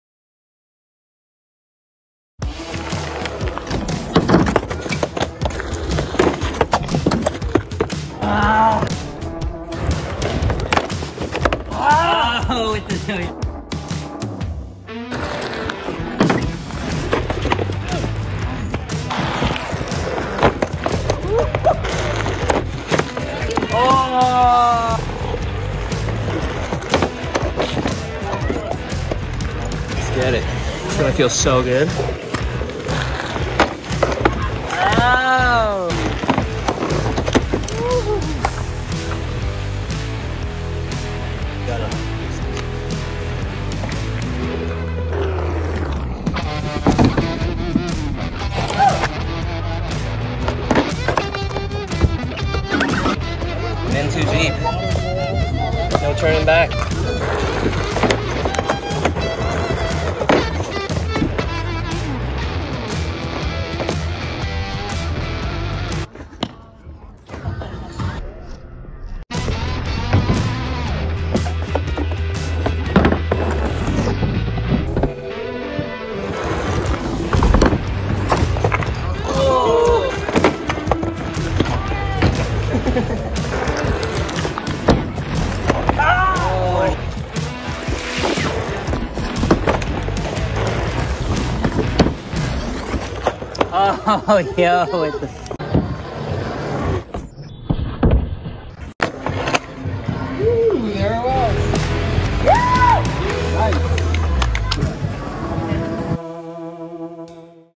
My War: Bonk back tail-slide
2 hours later, walked with 2 seconds of glory. Go conquer your battles!! ✊🏻

Enjoy the struggle as I try a bonk back tail to fakie and drive myself insane lol. Still need to clean up the slide, but happy to finally get one ⚔️

Eventually everybody lost interest and goes on about their lives lol Can't blame em!